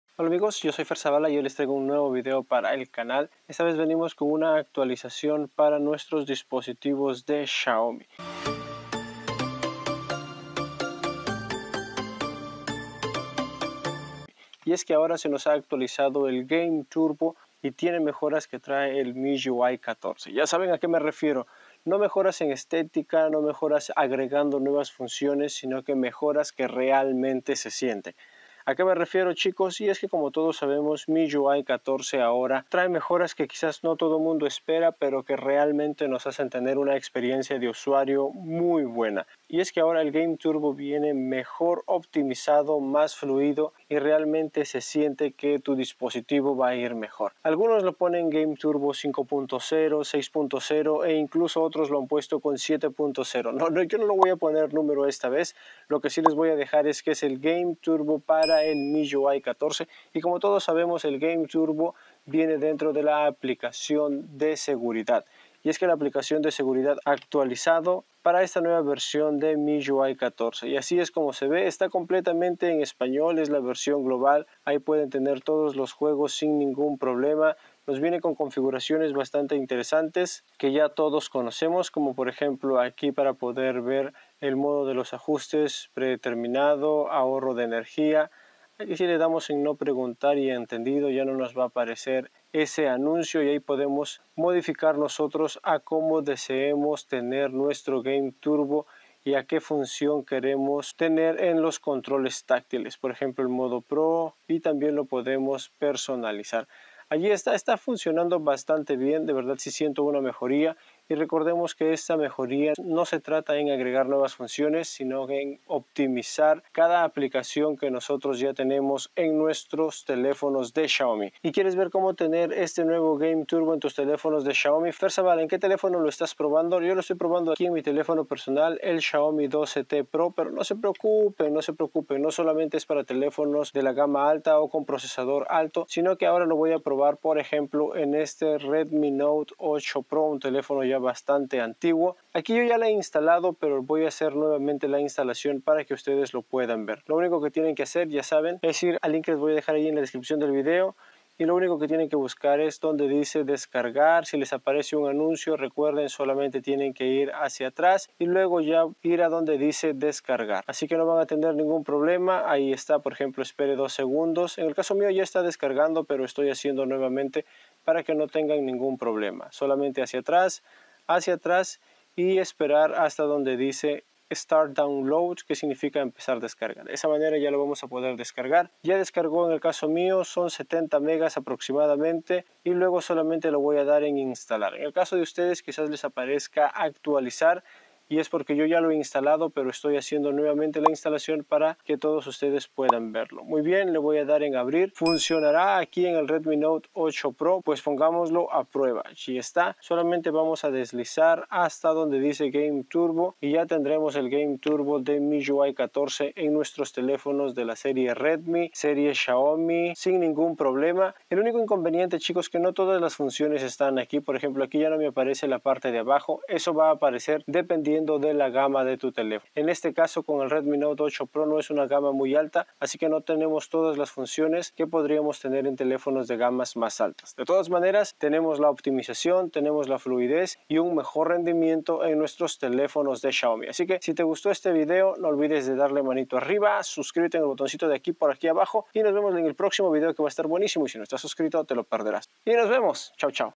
Game Turbo de MIUI 14 - EXCLUSIVO con Mejoras 🔥🔥🔥
Hoy Actualizamos el Game Turbo de nuestros teléfonos de Xiaomi, mira el vídeo para saber en que aspectos mejoro!
TIENES QUE PROBARLO